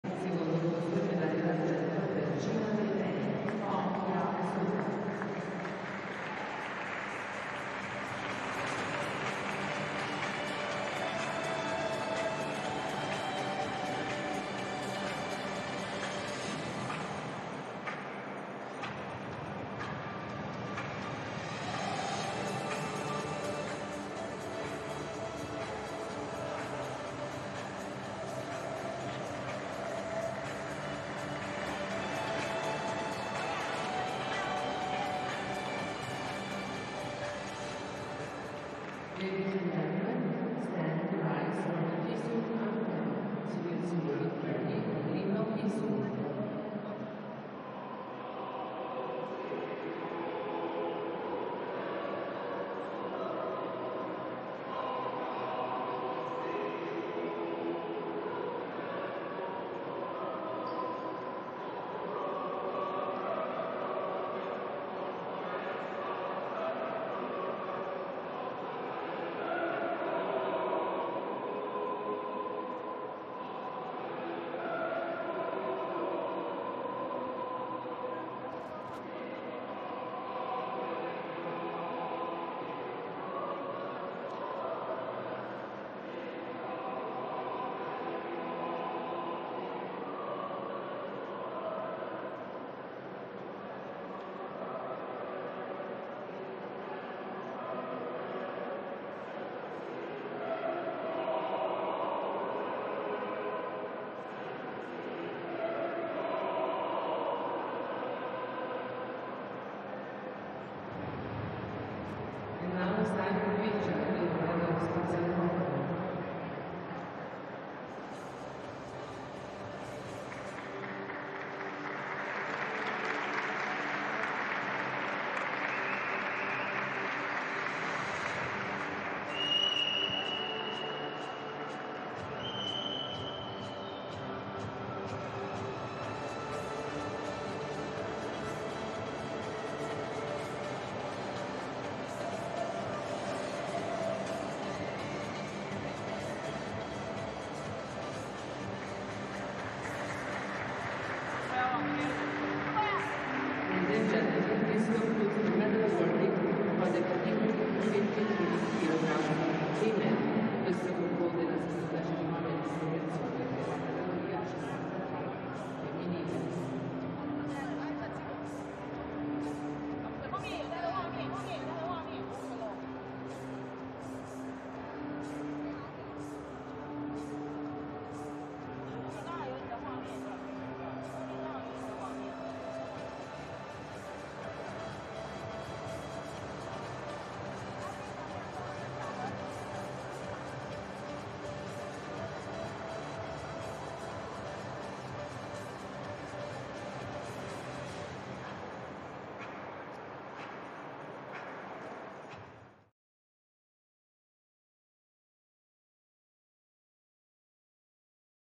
頒獎時刻::金牌:: 蘇柏亞 跆拳道 女子49-53公斤級決賽 全記錄 2019拿坡里世大運
轉貼▶ 請tag我們或註明來源為FISU
FISU - International University Sport